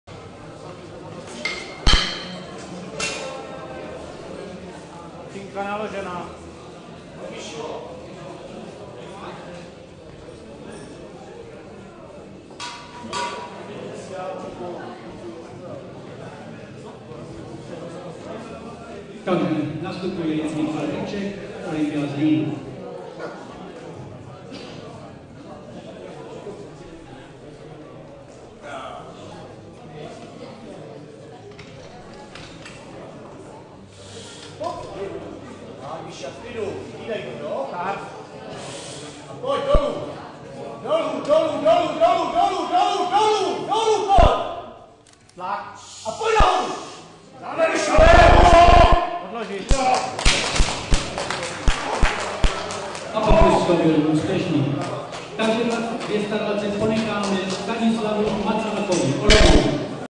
Krček Michal (1978), BP2, 220 kg
24. Mistrovství Jižní Moravy v silovém trojboji mužů a žen

Místo: Hornický dům, Zbýšov